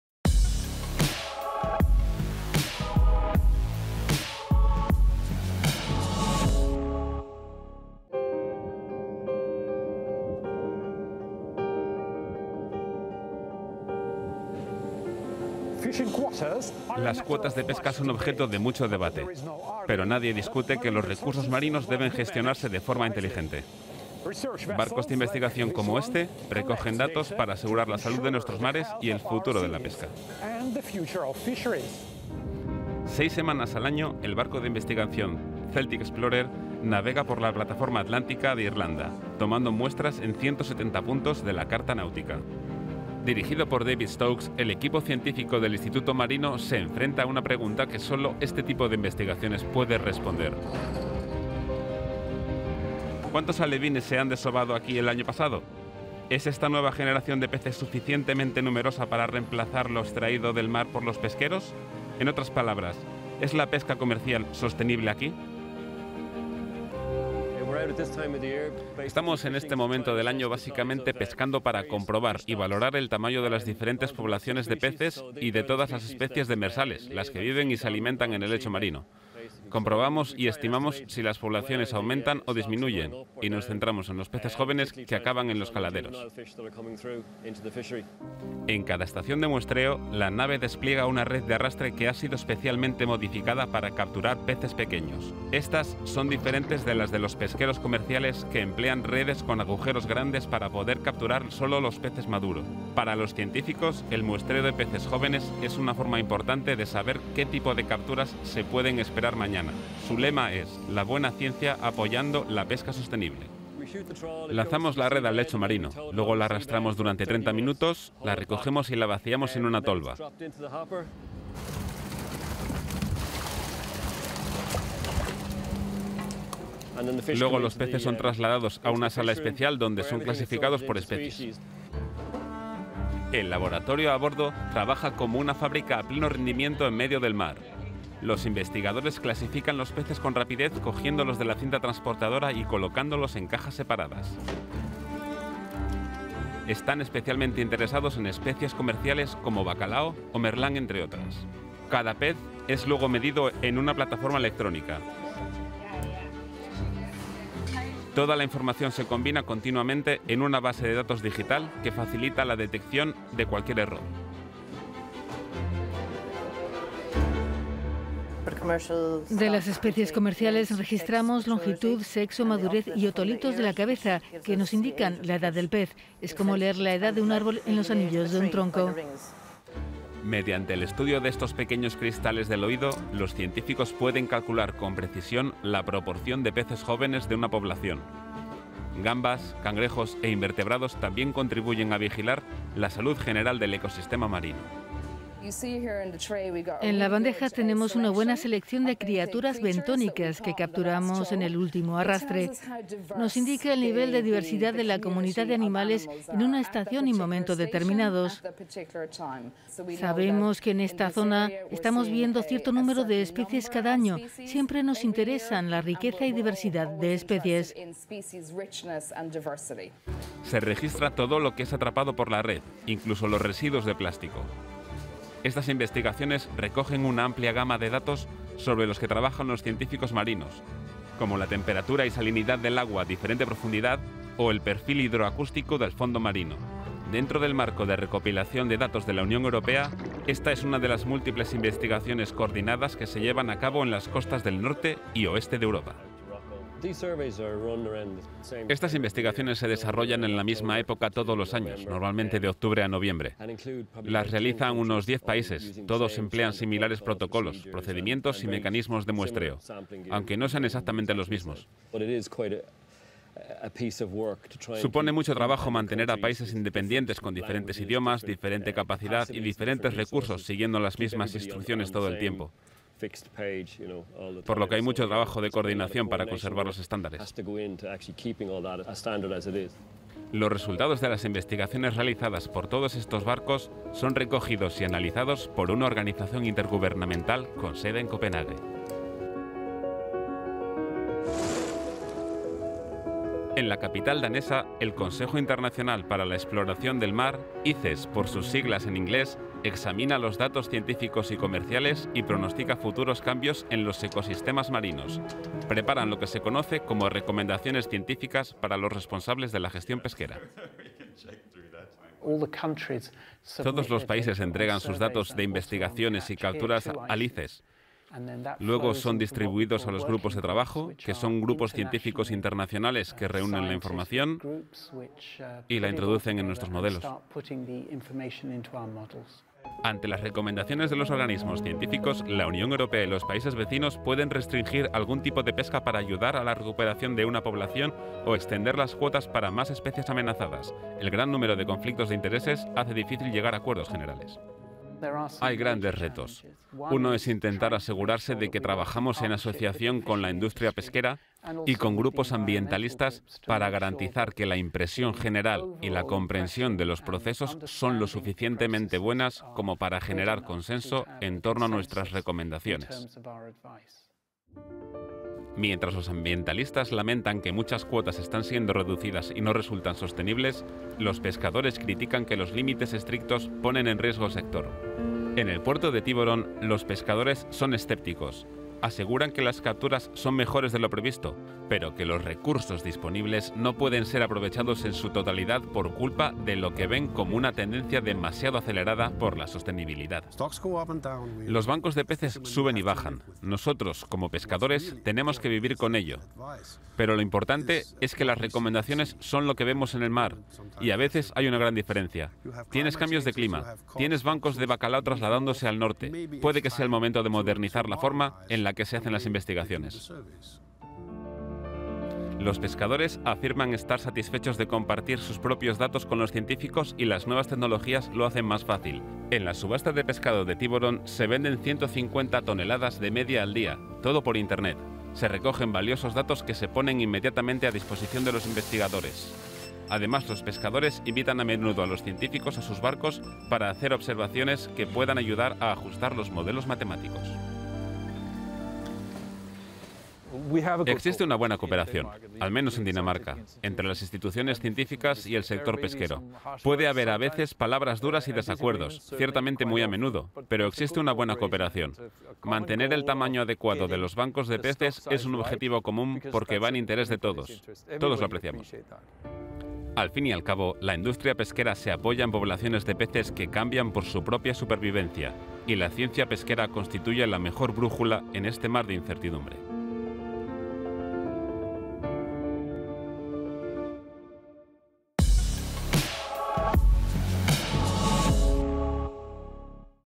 ¿Es sostenible seguir pescando al ritmo actual?Investigando el fondo marino para buscar la respuesta
Investigar en el fondo marino, clave para asegurar el futuro de los mares y la pesca
Un programa europeo de investigación rastrea el fondo marino para conocer el estado y composición de los bancos de peces y así poder recomendar nuevas medidas en torno a las limitaciones de pesca. Hay muchos intereses en juego y el consenso entre ambientalistas y pescadores es complicado.